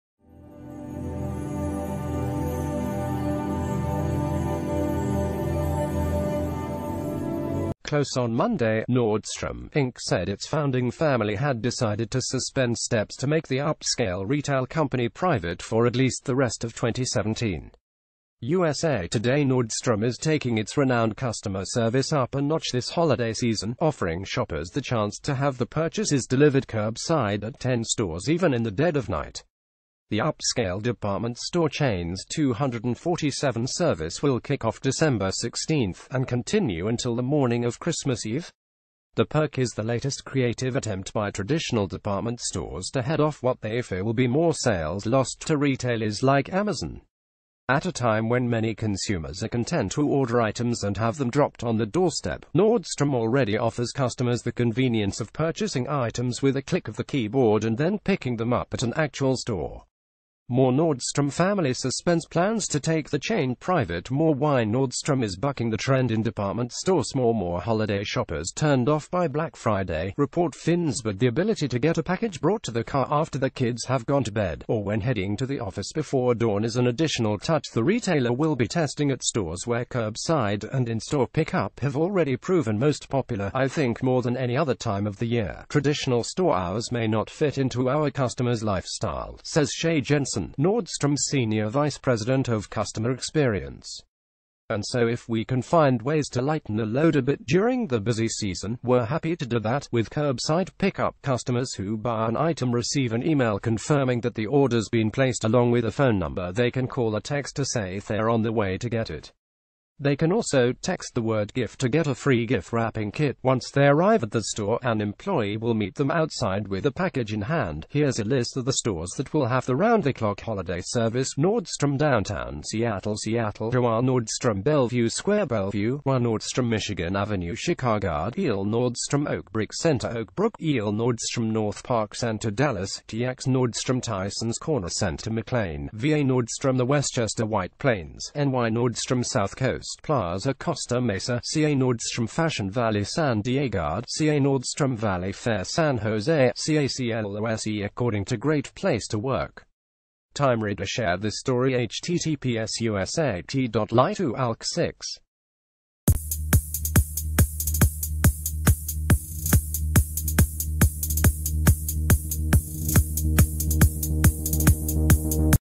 [Daily Times]Nordstrom to test 24/7 curbside purchase pickup at 10 stores
CLOSE On Monday, Nordstrom, Inc. said its founding family had decided to suspend steps to make the u...